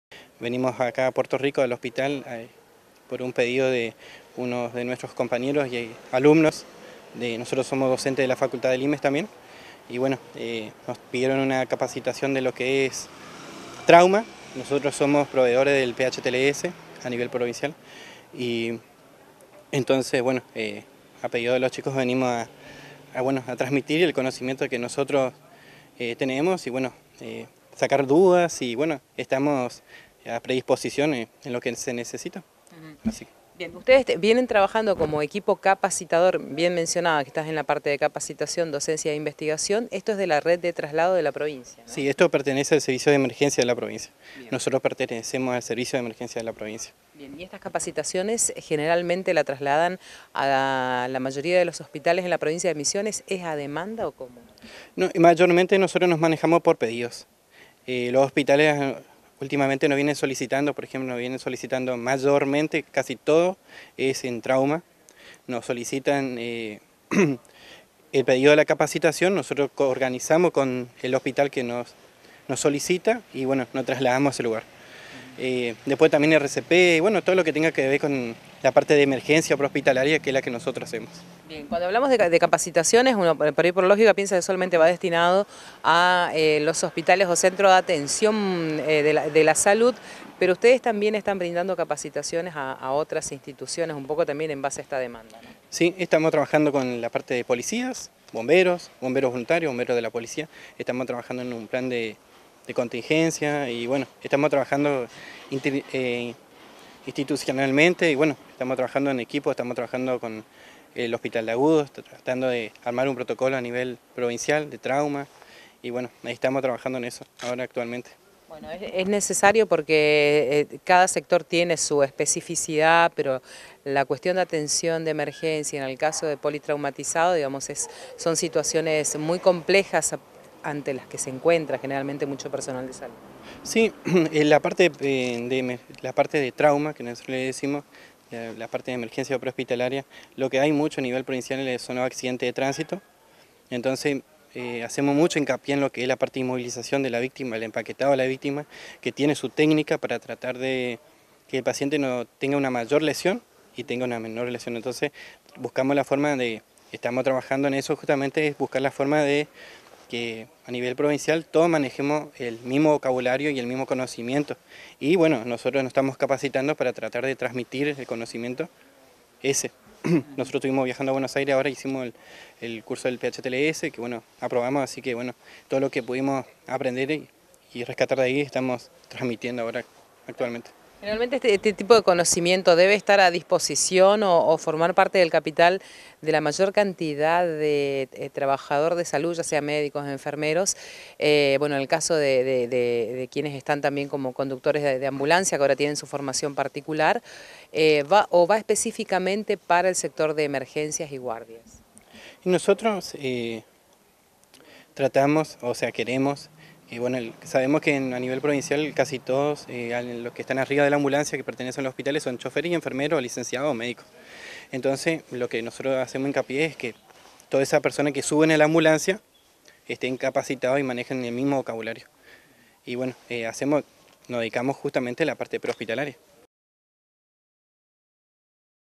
El Servicio de Emergencia desarrolló una capacitación en el Hospital de Área de Puerto Rico
La jornada de atención pre hospitalaria se llevó a cabo en el Hospital de Área Ricardo Gutiérrez de Puerto Rico y estuvo a cargo del Área de Capacitación y Docencia de la Red de Emergencia de la Provincia.